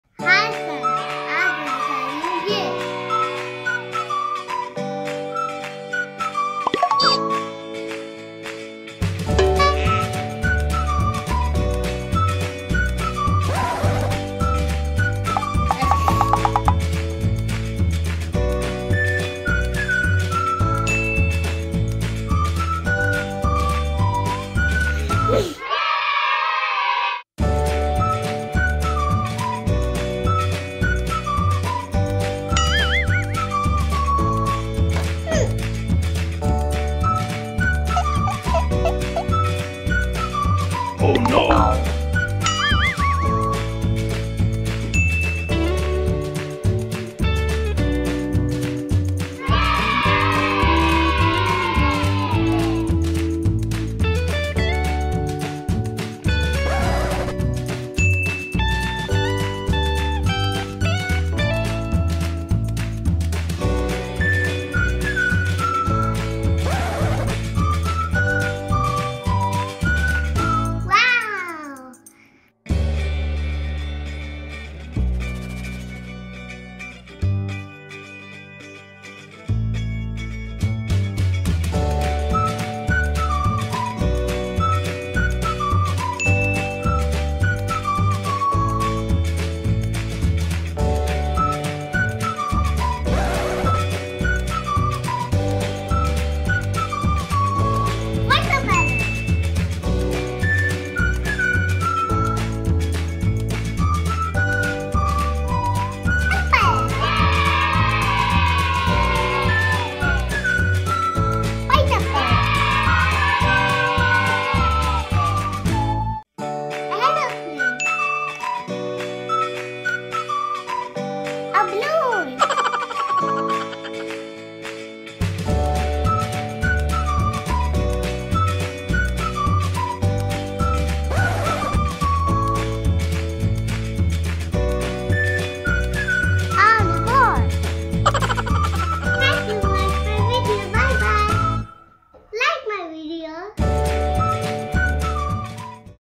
Savira Plays With Minions Projector Painting - Fun Toys For Kids
Funny Minions Painting Projector Set Unboxing & Playing